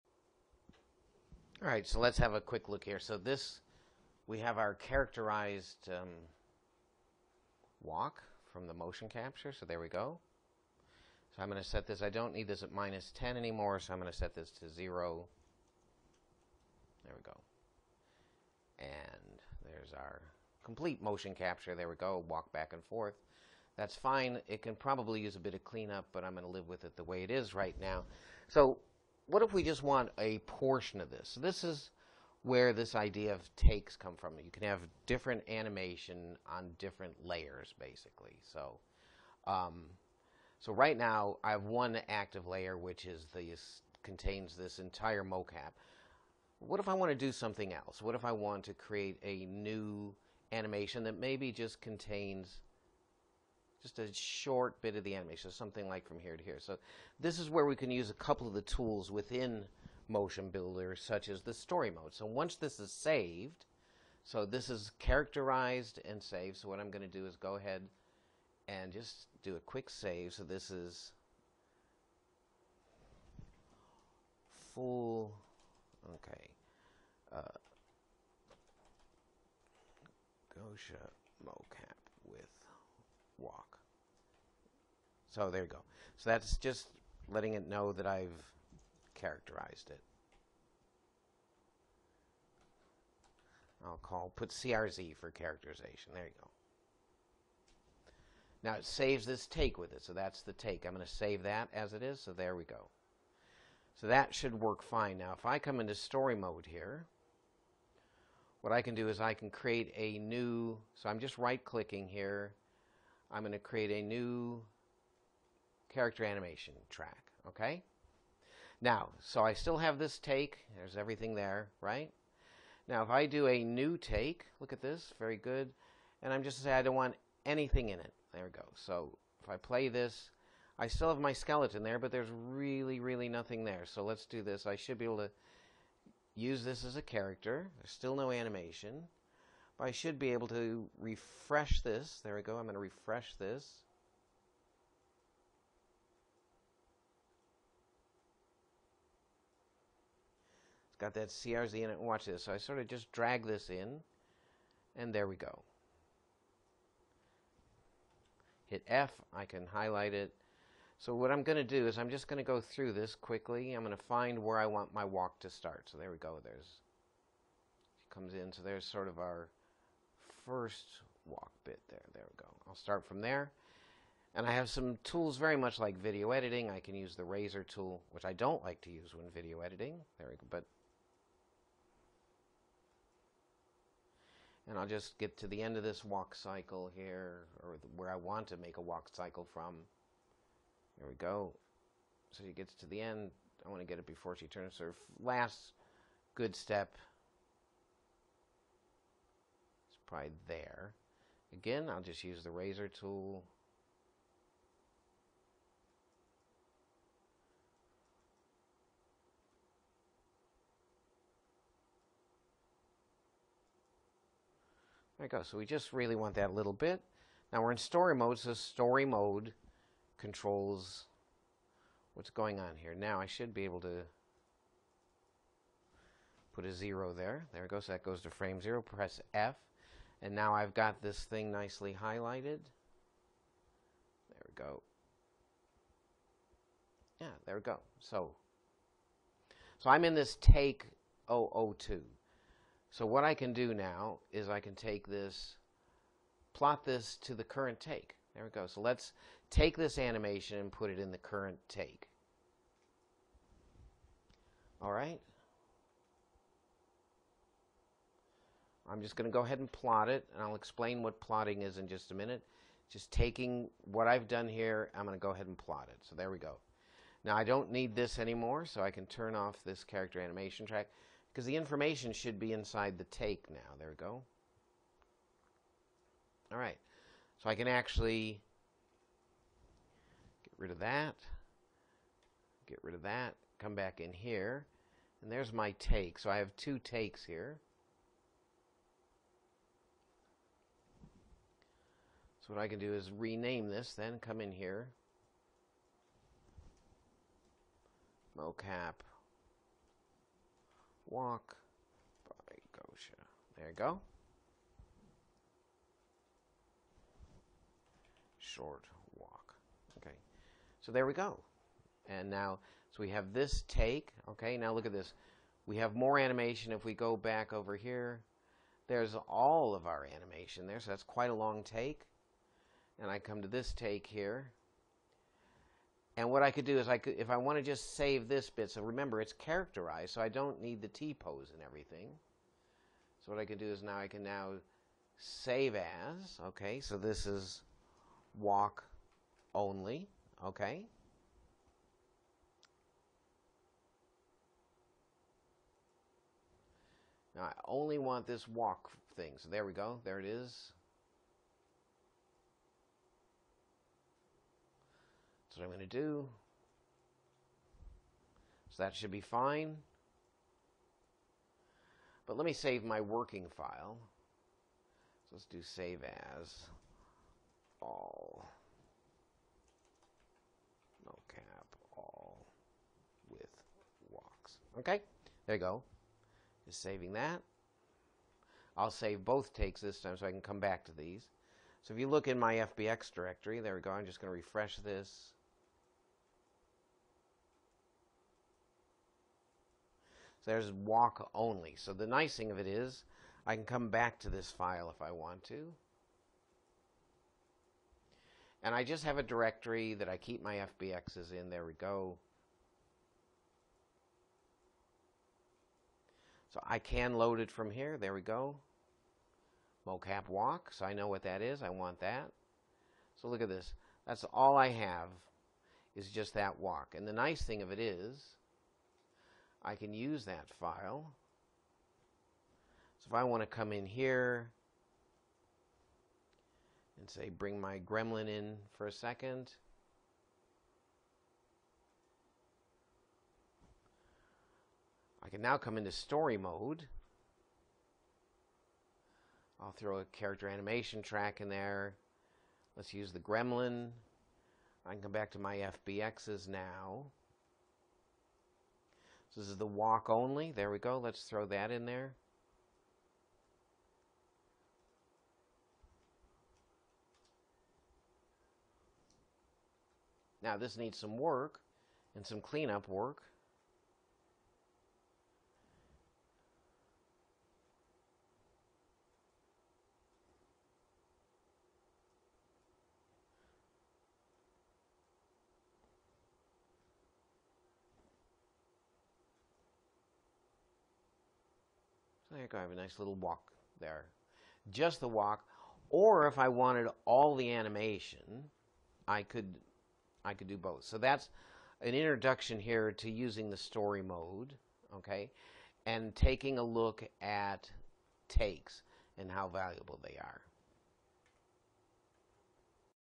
Edit motion capture data in MotionBuilder
Basic editing of motion capture data. Introduction to Takes and Story Mode in MotionBuilder